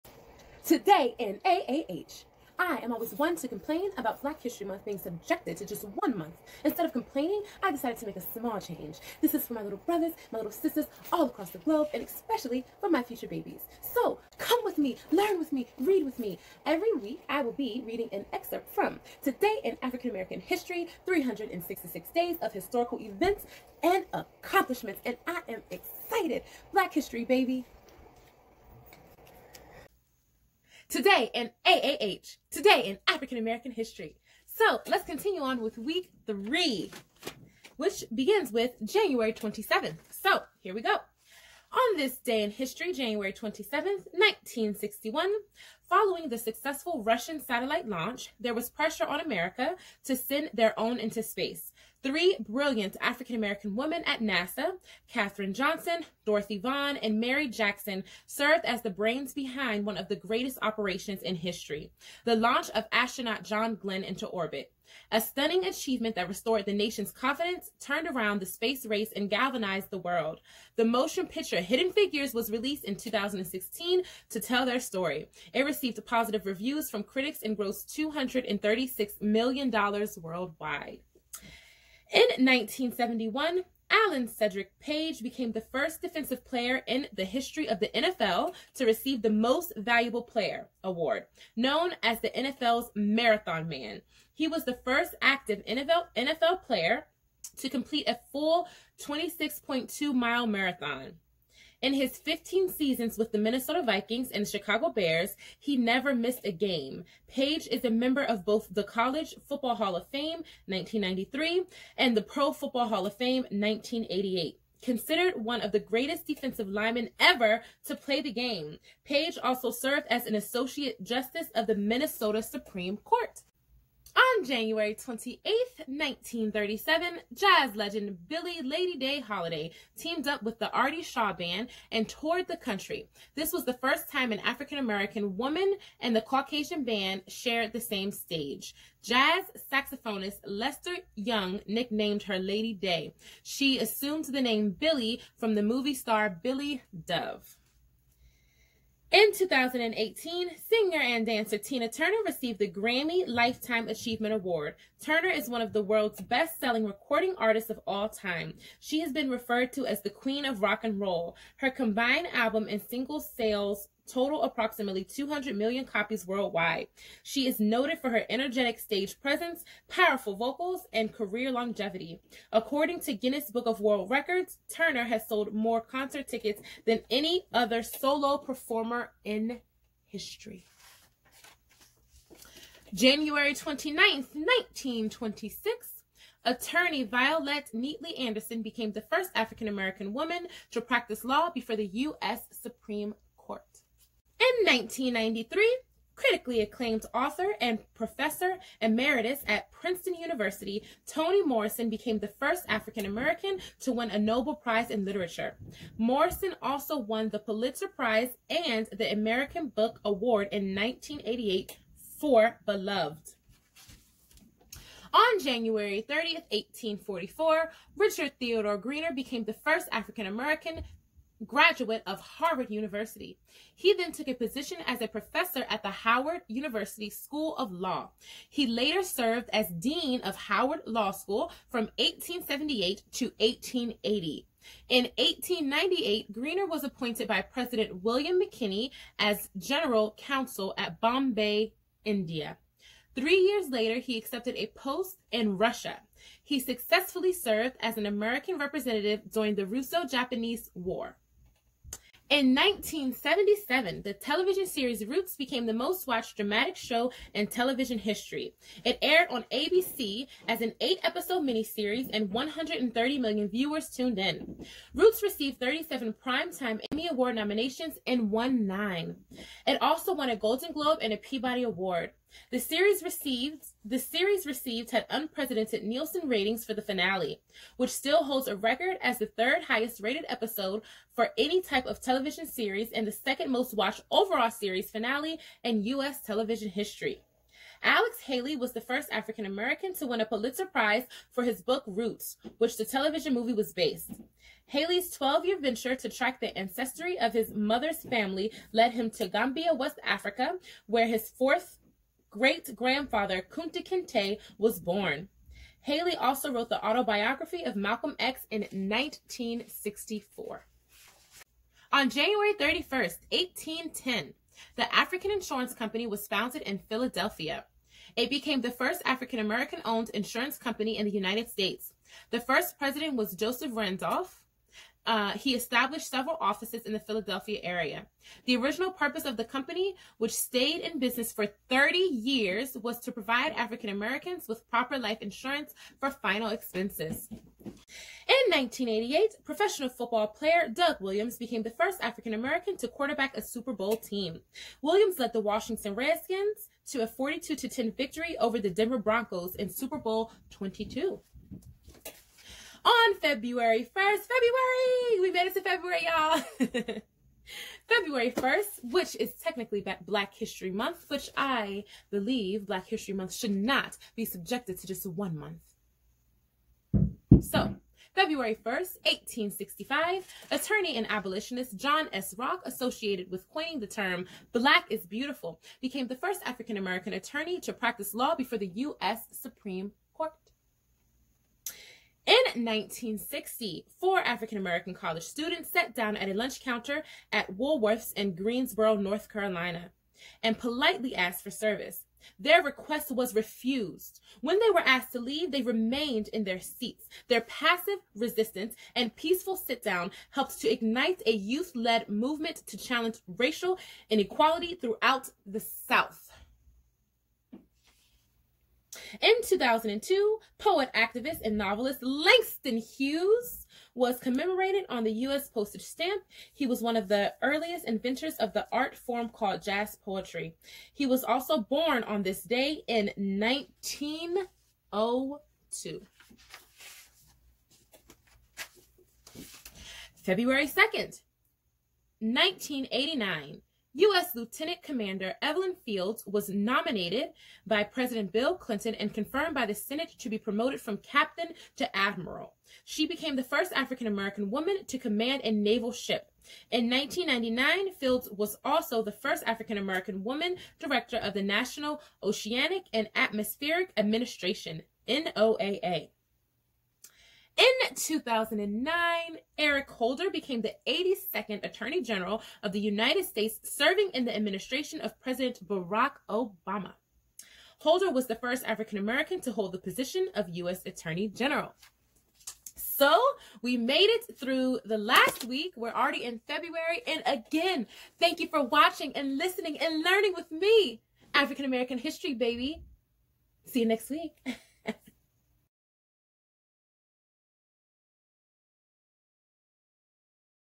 Episode 3 - Today in African American History with Queen Seekina
Come with me... Read with me... Learn with me! Each week for the duration of the year I will be reading an excerpt from "Today in African American History." Created so that my little brothas, sistahs and future babies can have the access to learn about AAH all year around... feel free to enjoy.